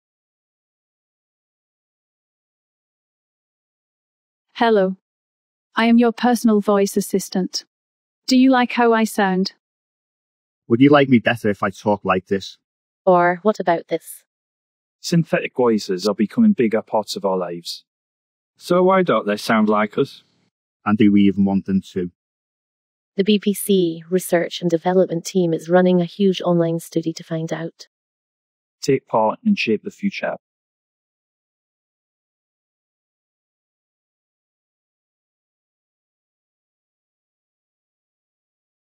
Synthetic voices: How do they make you feel?
How do audiences in 2020 feel about the use of synthetic voices in different types of media content ranging from national news to entertainment? Do regionality and gender affect the way BBC content can be perceived if delivered by synthetic voices? How would that make people feel?

BBC Research & Development have just launched an online study called Synthetic Voice and Personality, which tests several bespoke synthetic voices with British regional accents on a wide public audience.